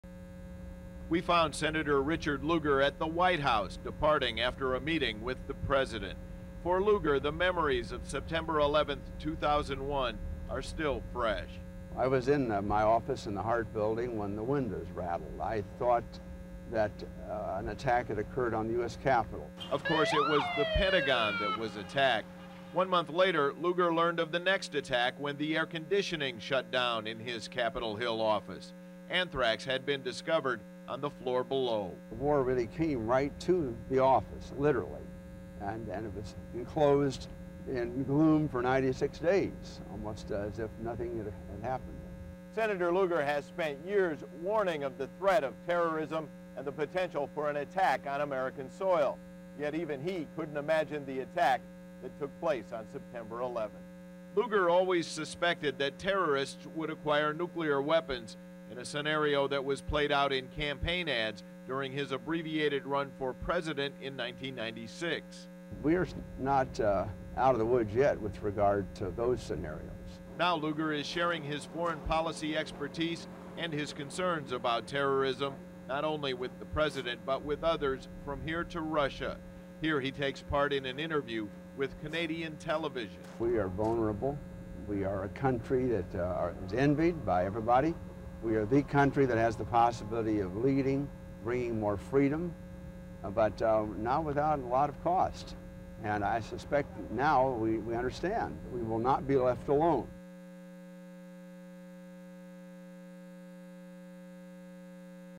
Sen. Richard Lugar shares memories of 9/11 in 2002
From WISH-TV on Sept. 11, 2002.

Reporter: Jim Shella.